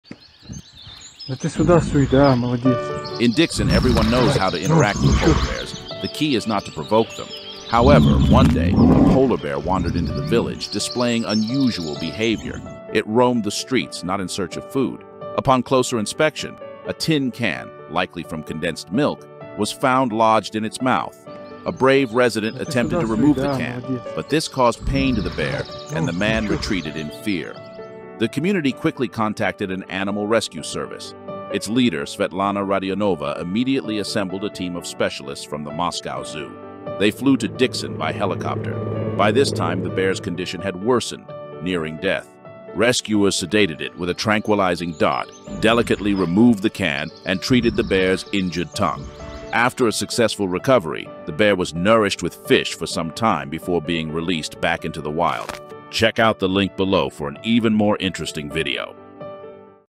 Bear Seeks Human Help! 🐻 😲
In Dixon, locals had a shocking encounter with a polar bear, but not for the usual reasons. Roaming the streets, this bear wasn't after food. Instead, a milk tin can was lodged in its mouth. A risky attempt to help ended in retreat. But hope wasn't lost! Svetlana Radionova and her team from Moscow Zoo flew in, battling time and the bear's deteriorating condition. Their valiant efforts led to the bear's rescue and rehabilitation. Discover this heart-stirring tale of human-animal cooperation.